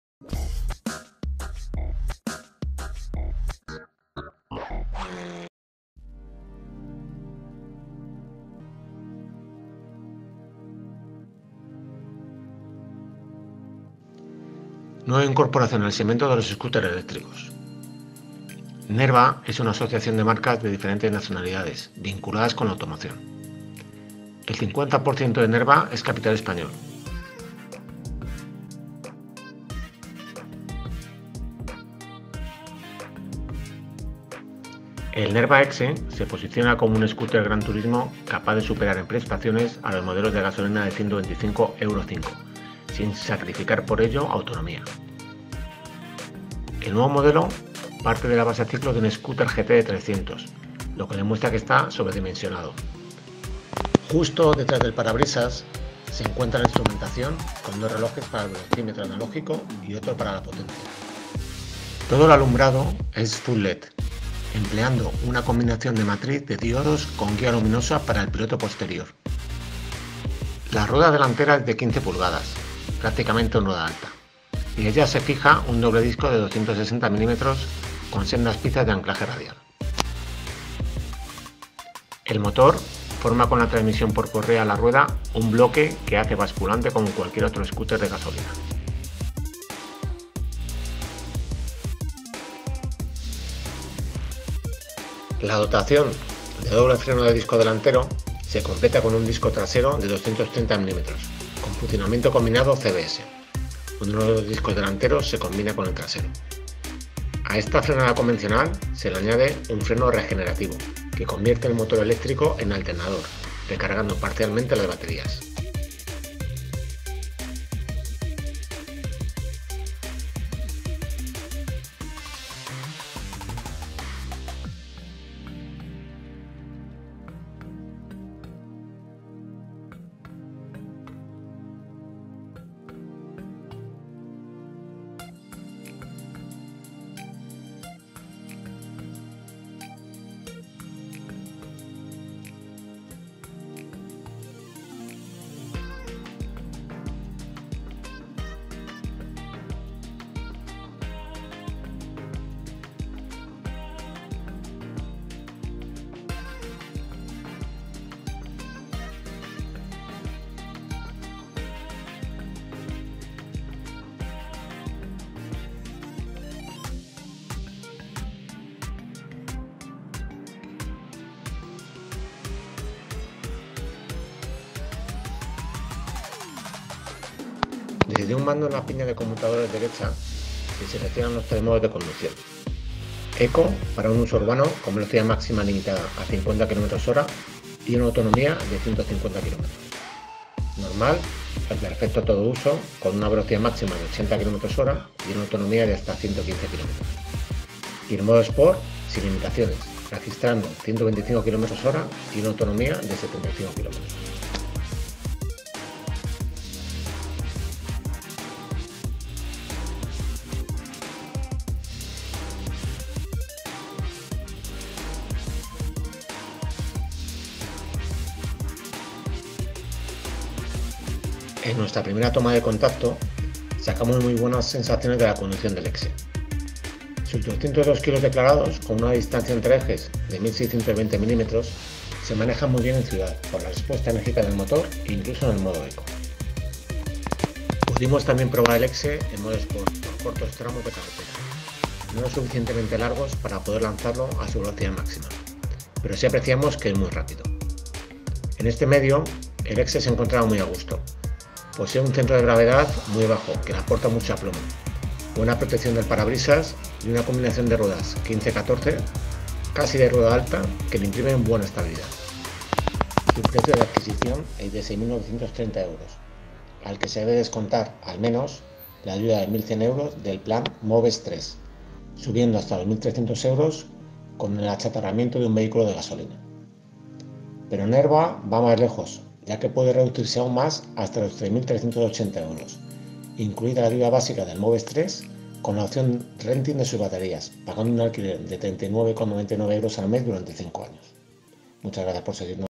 Primer contacto - Nerva Exe
Una nueva marca, Nerva,  irrumpe en el mercado de lo movilidad sostenible a través de una asociación de marcas especializadas en el mundo de la automoción con una participación española del 50% y en la que forma parte también BYD, firma líder en la producción de baterías y vehículos eléctricos. Su estreno se materializa con el scooter eléctrico Gran Turismo Exe, un modelo Premium que no defraudará al usuario que requiera tanto prestaciones como autonomía con el carnet de coche.